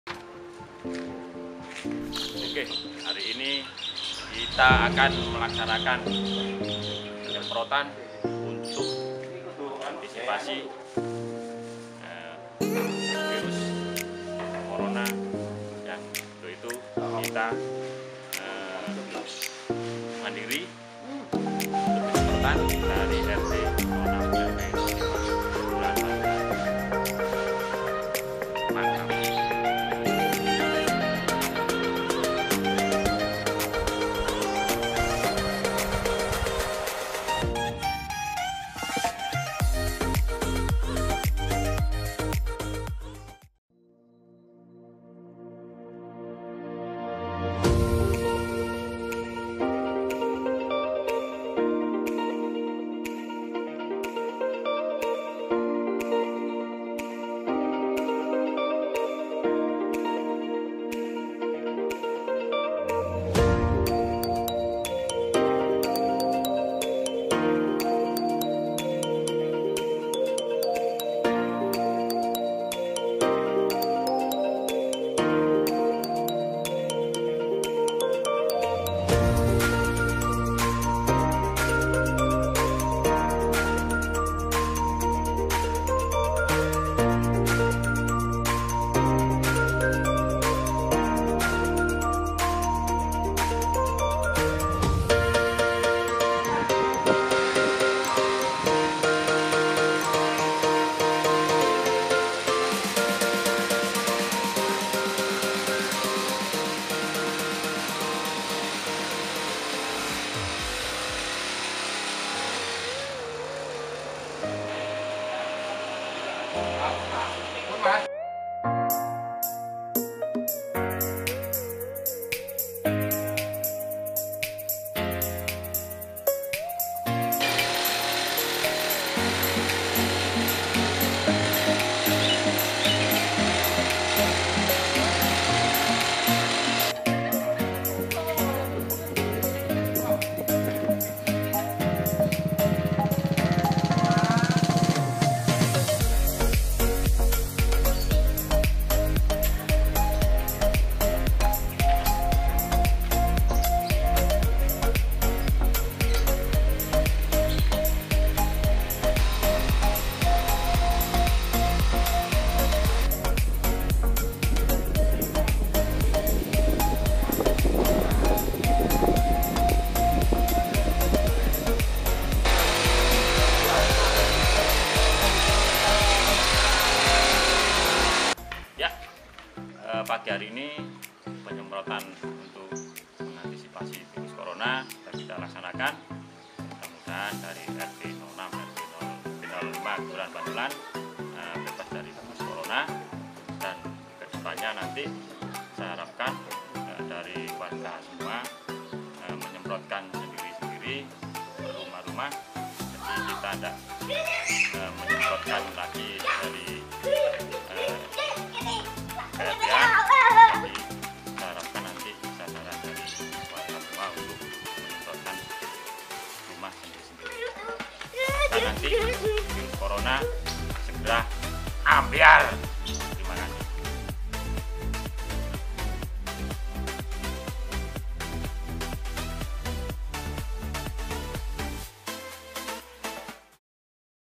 warga peduli
Para warga RT. 06 RW. 05 Kel bandulan bergerak bersama secara mandiri untuk menanggulangi penyebaran virus cororna dengan mengadakan penyemprotan secara masal di lingkungan RT dengan swadaya. semoga warga RT. 06 RW. 05 Kel Bandulan selalu sehat. Amin